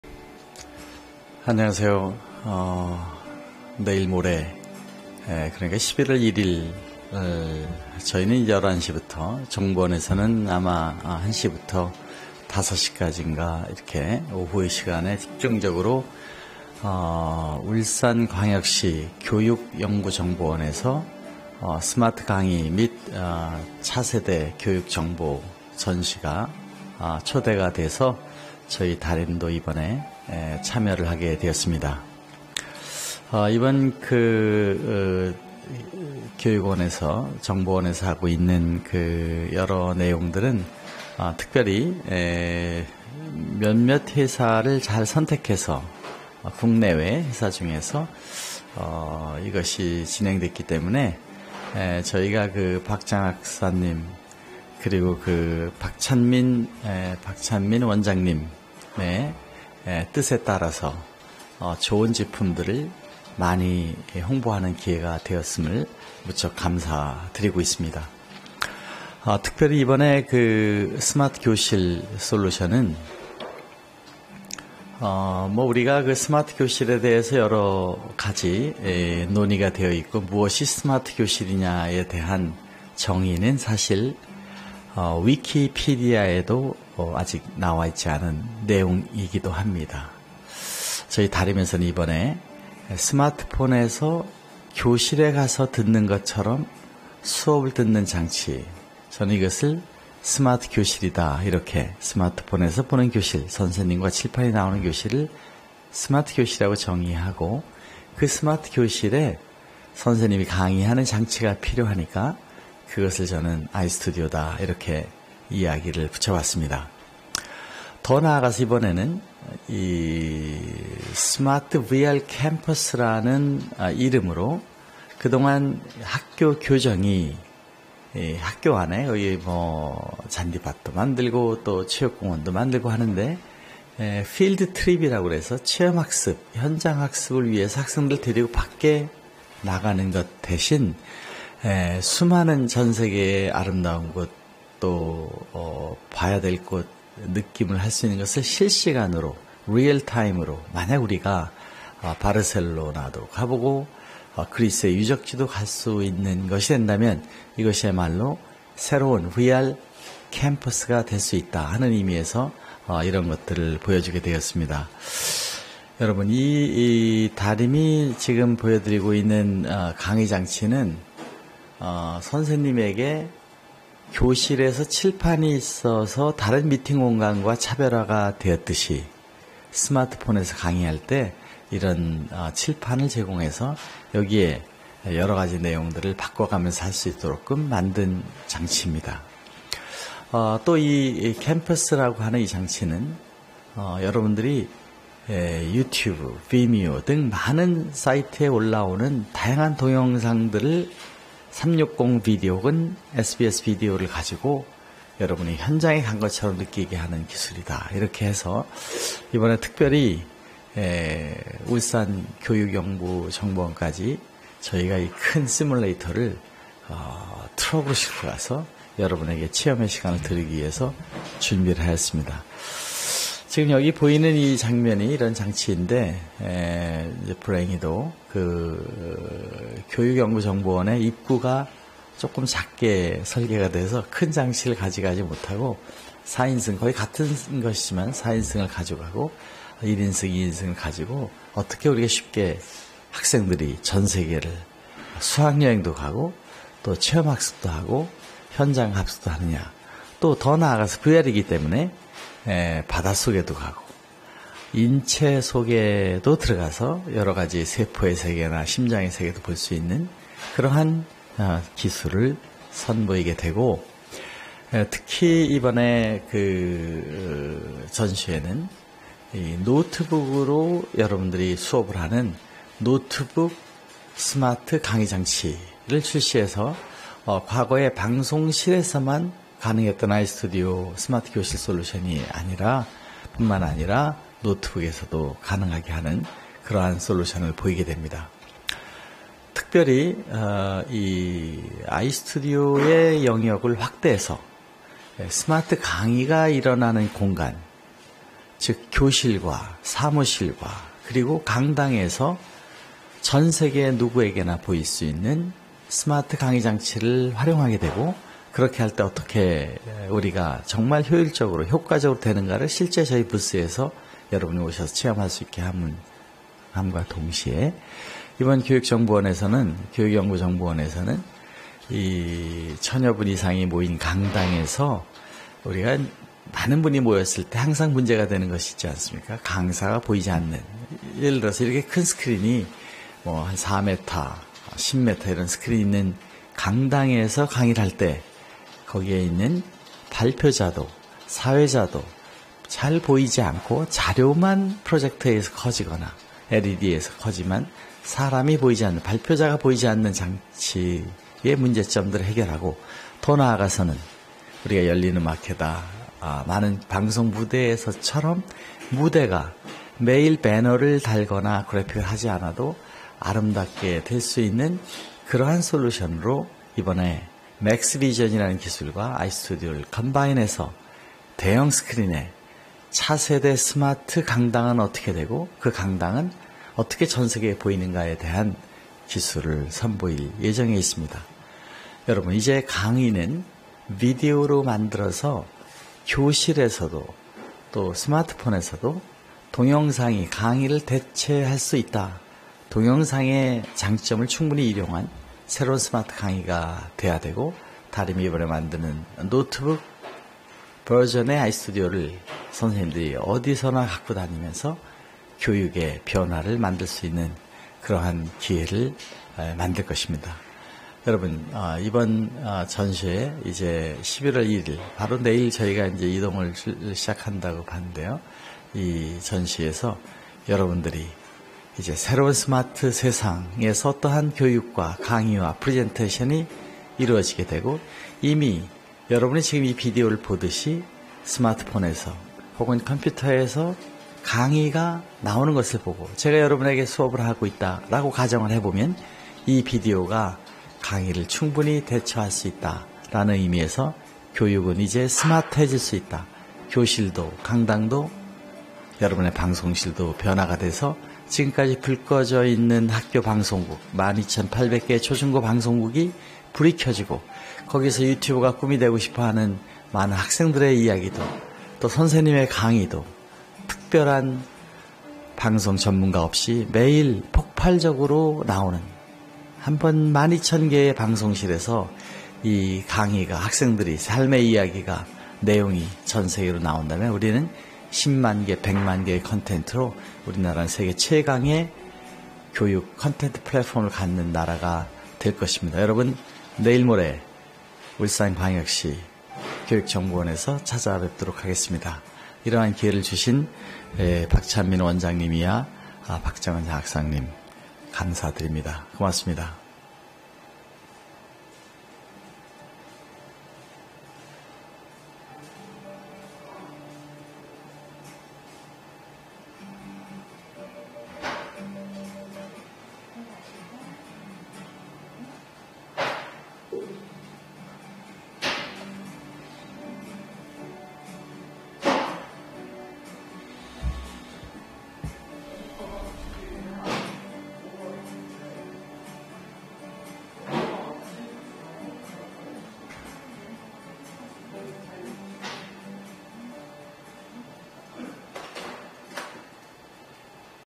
스마트 강의 장치 iStudio 시스템들이 울산 광역시 교육 정보원에서 선보입니다.
iStudio 는 실시간으로 교실서 사무실서, 전세계 스마트 폰으로 실시간 강의를 하는 스마트 시대의 강의 장치입니다. 이번 11월 1일 울산광역시교육정보원 초청 전시회 및 발표에선 iStudio 스마트 강당 기술과, 전세계 어느 곳에서도 현장학습, 체험 학습을 할수 있는 VR Campus 의 TeleCopter로 독도, 제주도 수학여행을 체험할수 있게 하고 있습니다.  울산광역시교육정보원 스마트 교육 정보 전시회에 초청을 해주신 박찬민 원장님, 박정은 장학사님과 담당 선생님들께 감사드립니다.  여러분의  많은 방문 바랍니다.